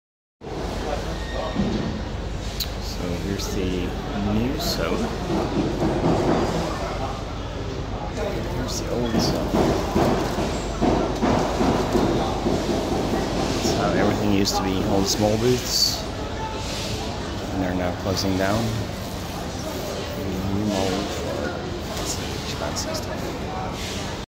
Shenzhen - out with the old. In with the new and shiny?
Looks like several of the Shenzhen component markets are in for an change. Especially visible right here where Chinese made IoT makers with fancy shops are squeezing out old traders.